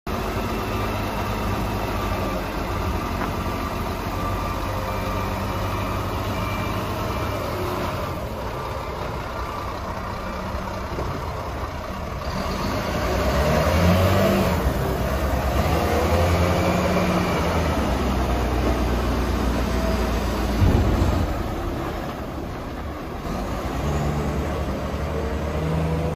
2002 VOLVO A40D For Sale
NEW ENGINE, TRANS AND DROP BOX, NEW HITCH & CENTER PINS, 85% RUBBER - EXCELLENT CONDITION